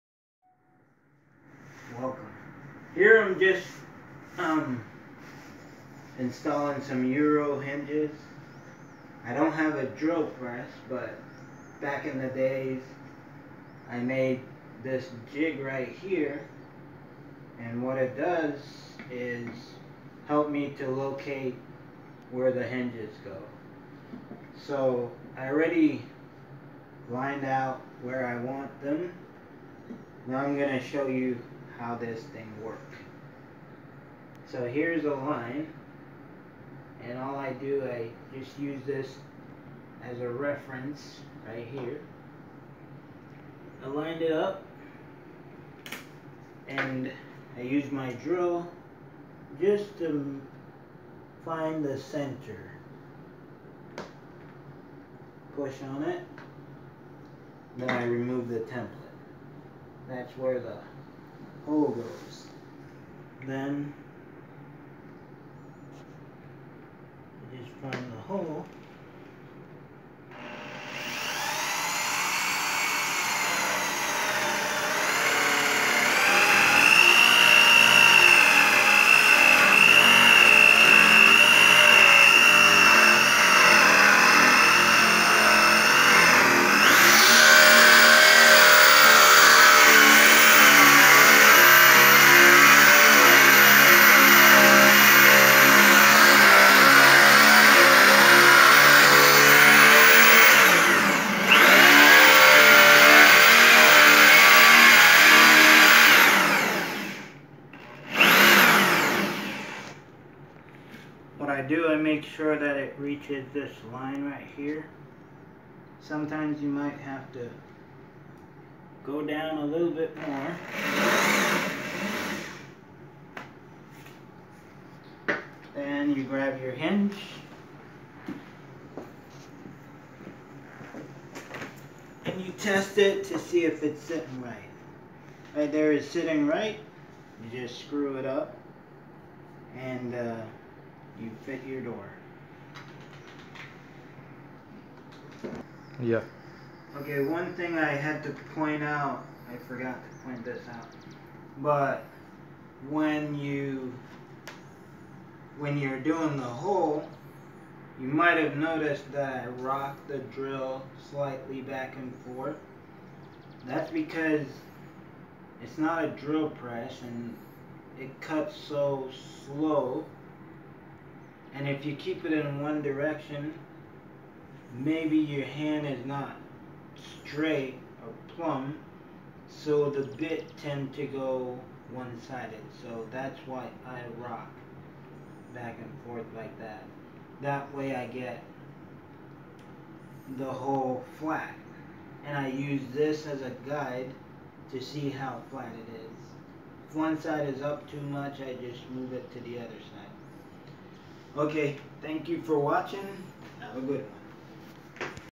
How to make holes for Euro hinge without a drill press
How to make holes for Euro hinges without a drill press